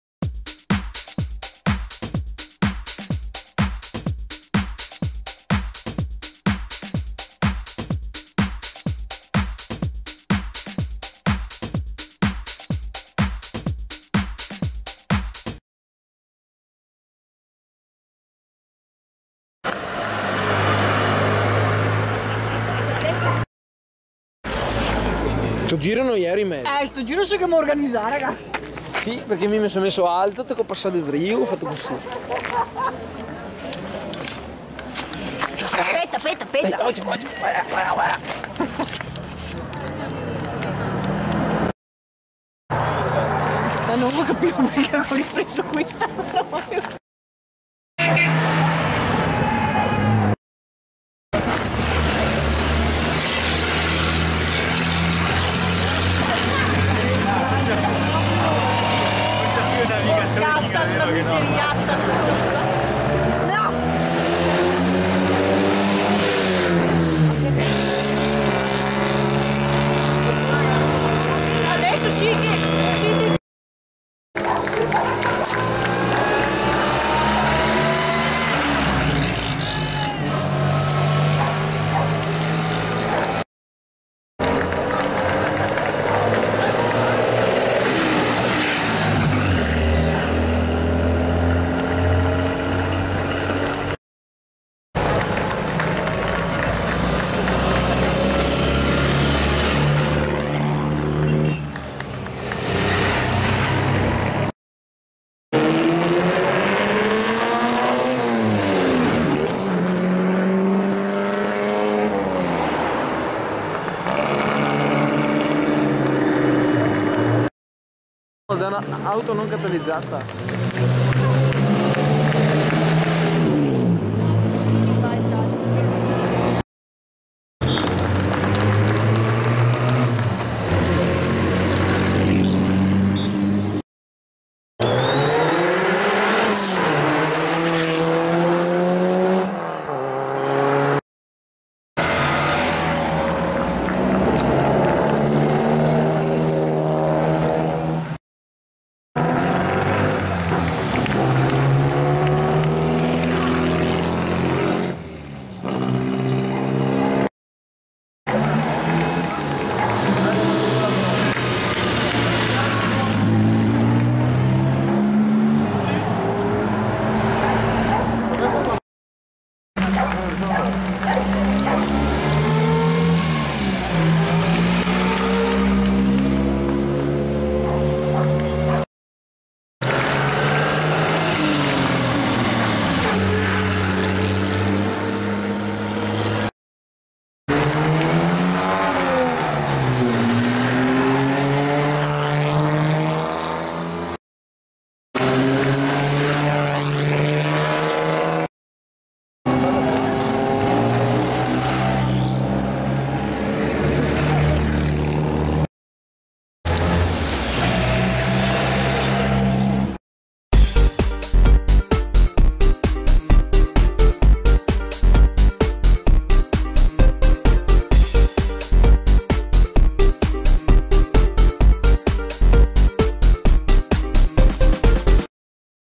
15 Maggio 2014 Mille Miglia Volume 19
Diciannovesima parte del Video realizzato durante la manifestazione "Mille Miglia" a Borgoricco